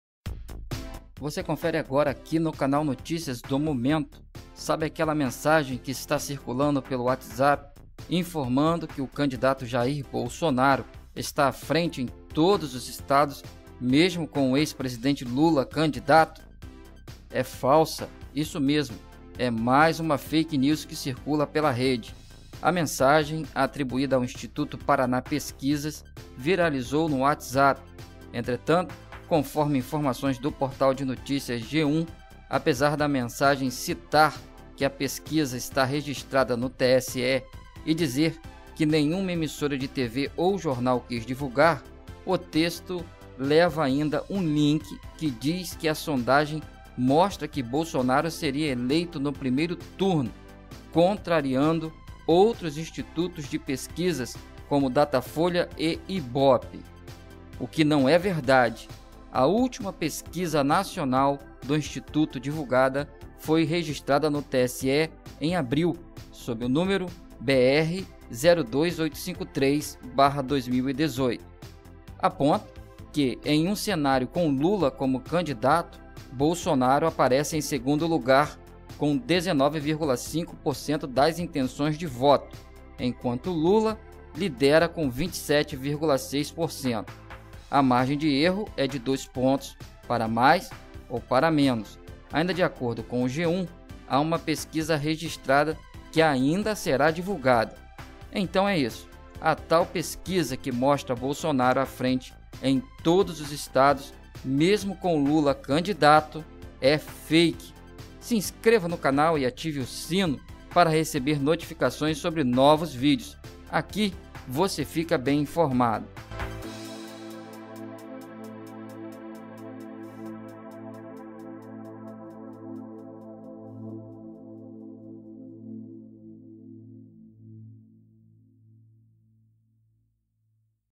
Pesquisa que mostra Bolsonaro à frente mesmo com Lula candidato é FAKE.
Sabe aquela mensagem que está circulando pelo WhatsApp informando que o candidato Jair Bolsonaro está à frente em todos os estados mesmo com o ex-presidente Lula (PT) candidato? É falsa!